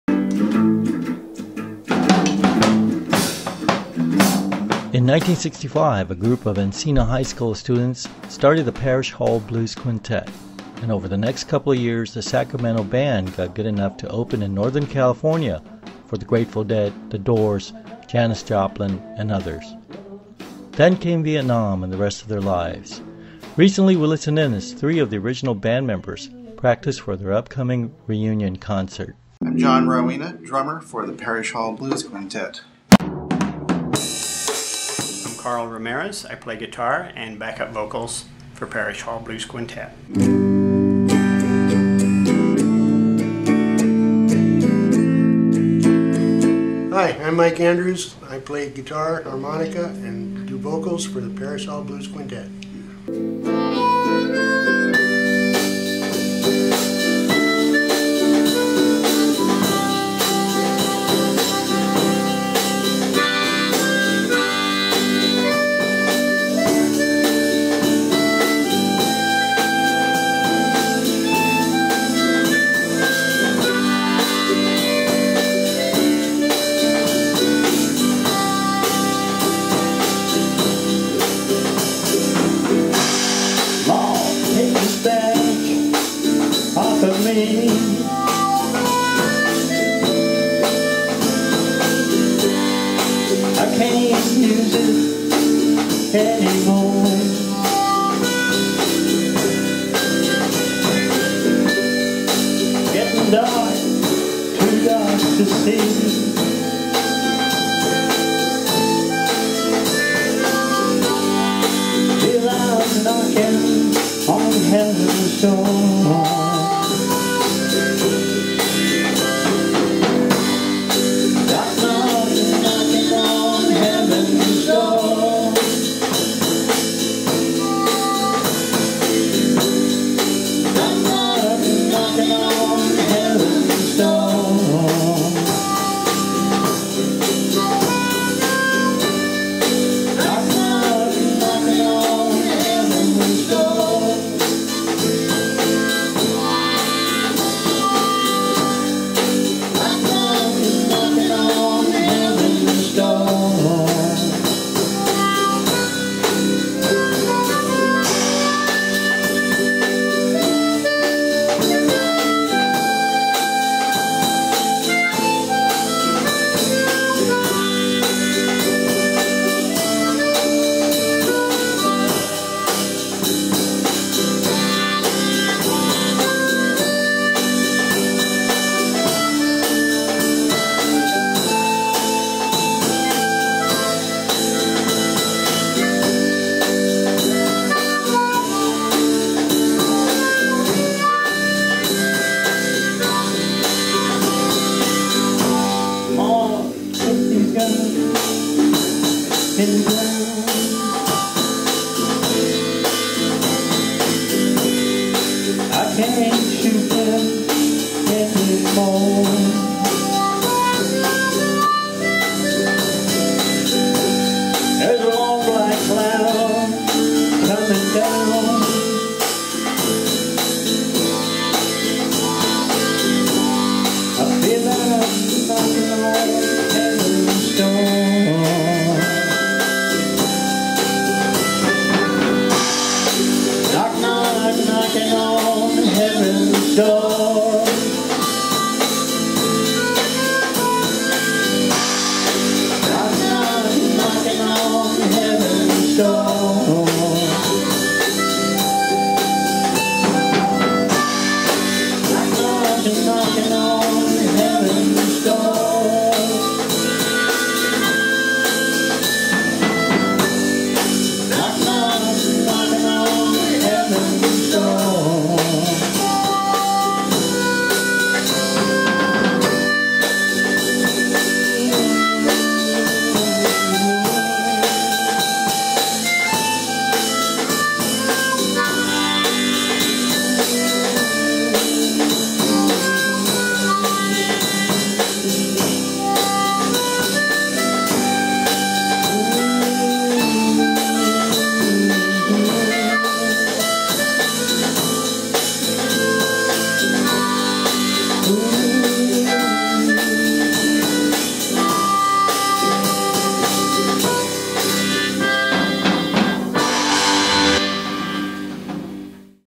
The Parish Hall Blues Quintet - the Sacramento band that opened for The Doors still playing
In 1965 a group of Encina High School students started  the Parish Hall Blues Quintet and over the next couple of years, the Sacramento band got good enough to open in Northern California for the Grateful Dead, the Doors, Janis Joplin and others. Then came Vietnam and the rest of their lives.
Recently we listened in as three of the original band members practice for their upcoming reunion concert.